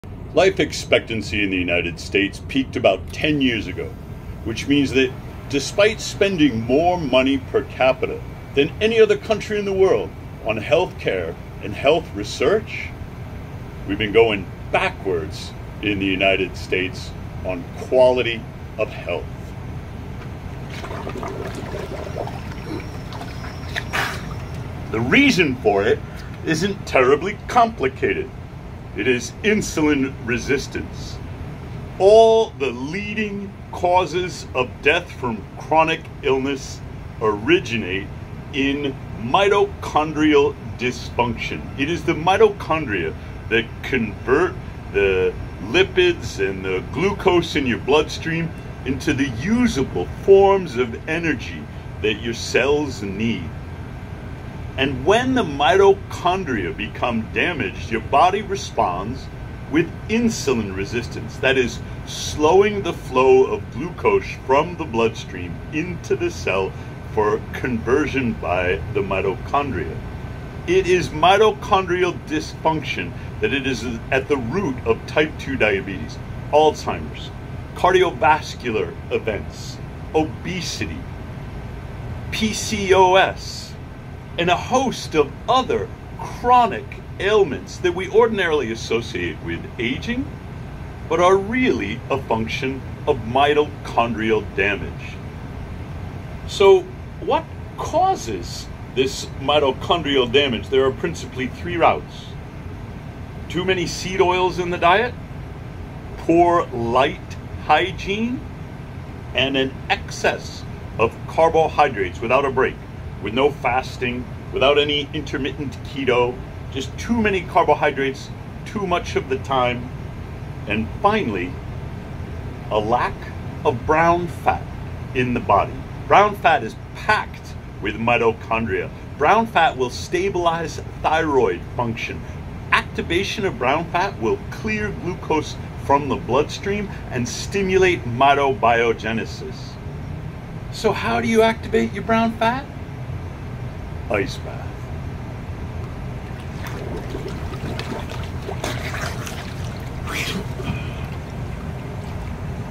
Insulin Resistance is the Body's Response to Mitochondrial Disfunction | Morozko Science
All the leading causes of death from chronic illness in the United States originate in insulin resistance -- and it's getting worse. American life expectancy peaked in around 2014 and has been declining ever since -- despite spending more money on healthcare and health research than any other country in the world!

Insulin resistance is the human body's response to mitochondrial dysfunction.

Too many seed oils, poor light hygiene, and excess carbohydrates in the diet without a break will all damage mitochondria. However, cold exposure stimulates restoration of mitochondria and activation of brown fat. Higher levels of brown fat are associated with metabolic health, whereas less brown fat is associated with higher rates of metabolic disorder.

The best way to activate your brown fat?

Ice bath.